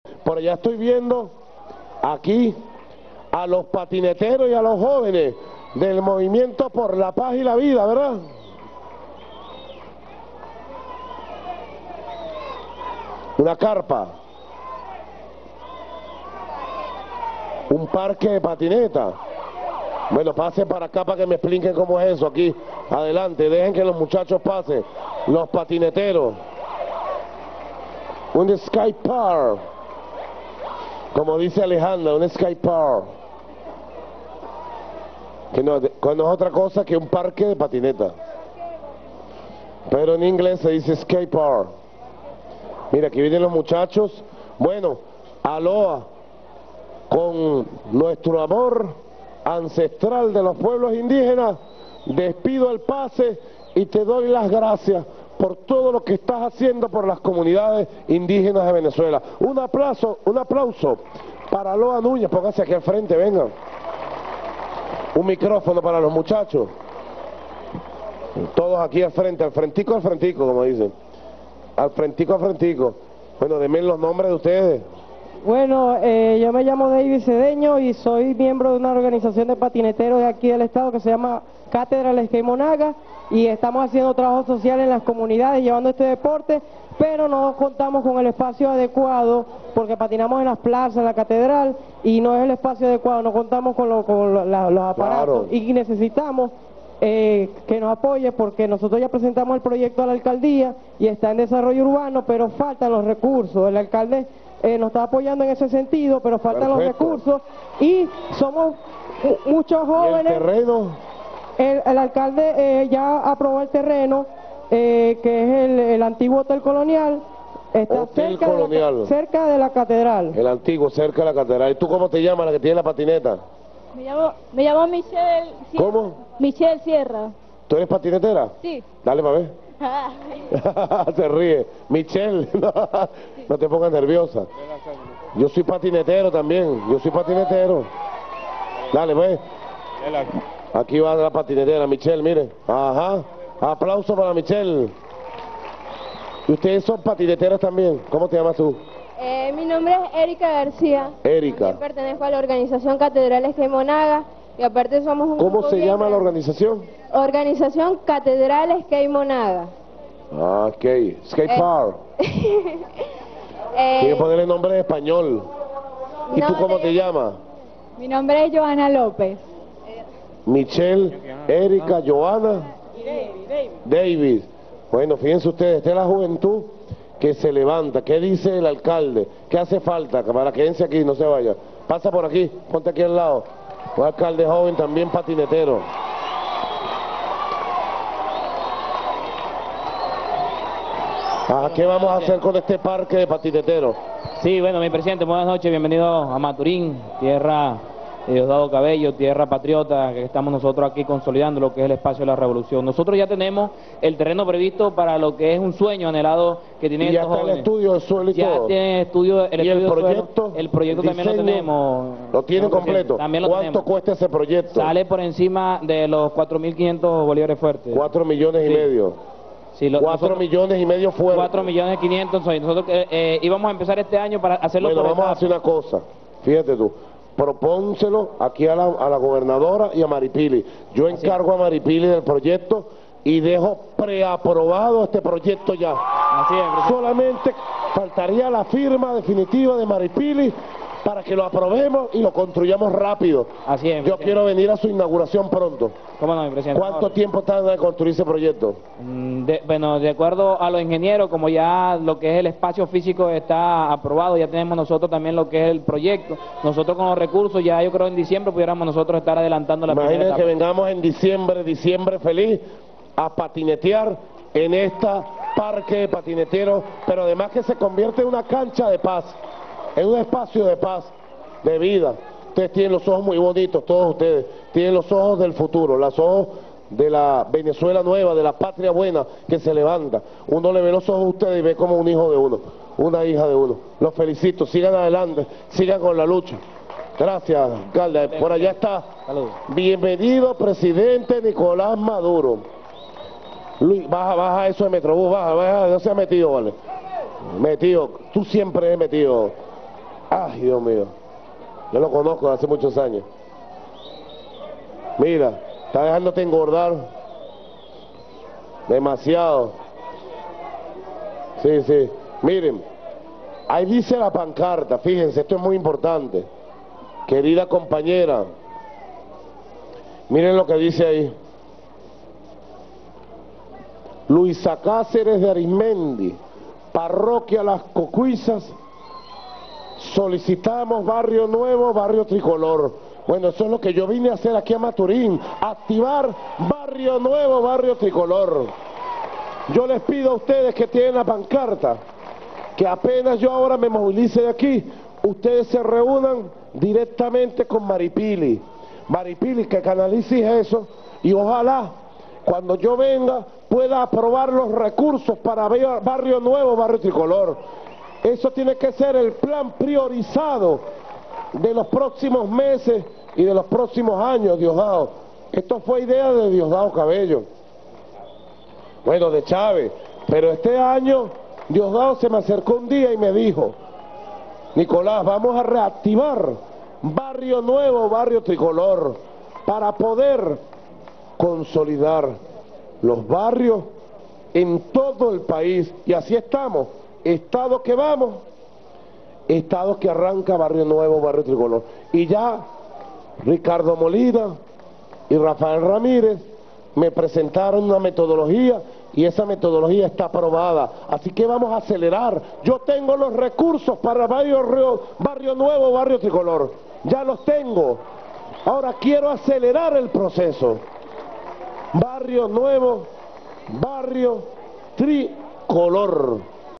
Nicolás Maduro: Yo también fui patinetero
El presidente venezolano, en visita de gobierno de calle en el estado Monagas, conversó con jóvenes de la entidad que le solicitaron la  construcción de un "skate park" o parque de patineteros, debido a que en el lugar no hay ningún sitio adecuado para practicar el deporte. El presidente manifestó que, en su juventud, también fue patinetero y se hicieron los arreglos para iniciar el proyecto.